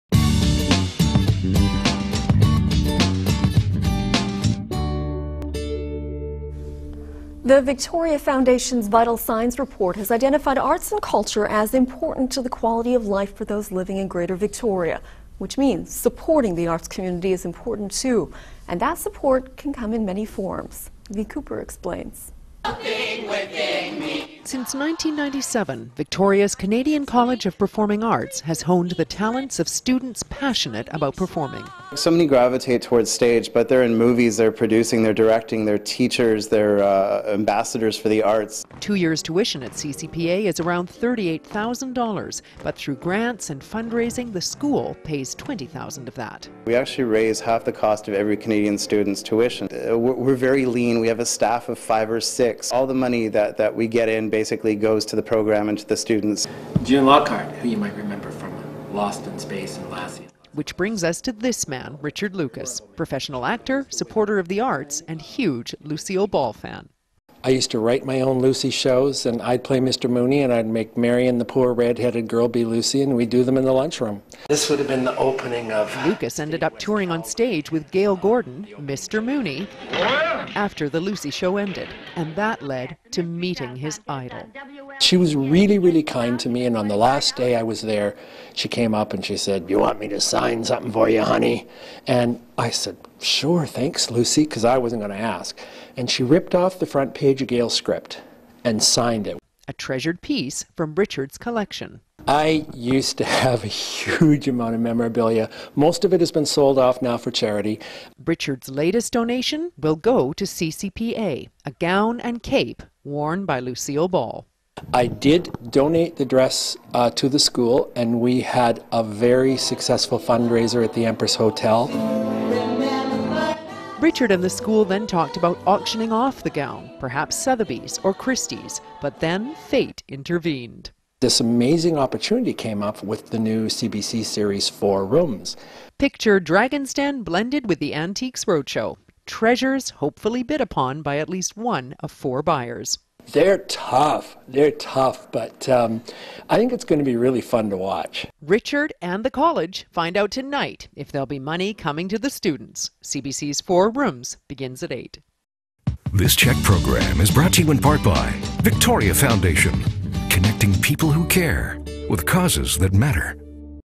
Vital People: CCPA & Lucille Ball's dress
Support of the arts can take many forms. One Victoria arts supporter, Richard Lucas recently took a dress once belonging to Lucille Ball to CBC's Four Rooms. If it sold, Lucas intends to donate the proceeds of the dress to the Canadian College of Performing Arts. Since 1997, the college has honed the talents of students who are passionate about performing.